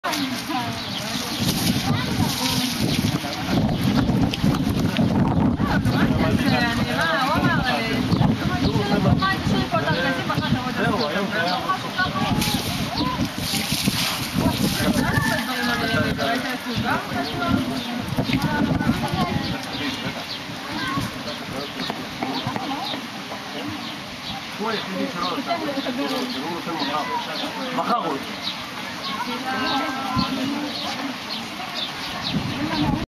Elephant self showering
elephant showering himself at the zoo in kiryat motzkin